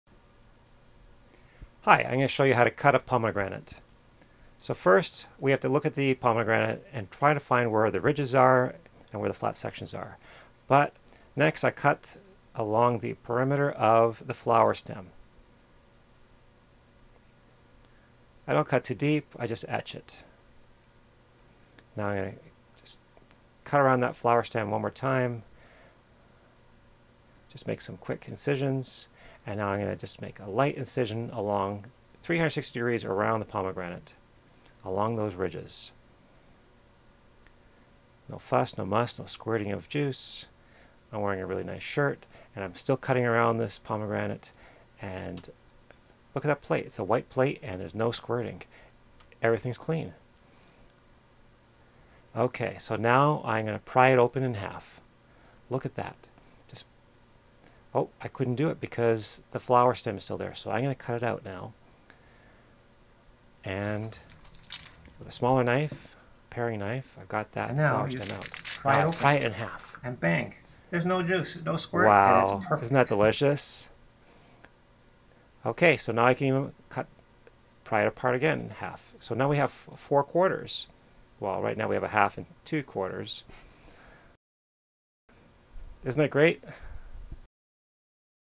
How to cut a pomegranate
David Michaels shows you how to cut a pomegranate into 4 neat quarters. No squirting, no mess, no fuss. Watch how I cut a pomegranate while wearing a nice shirt.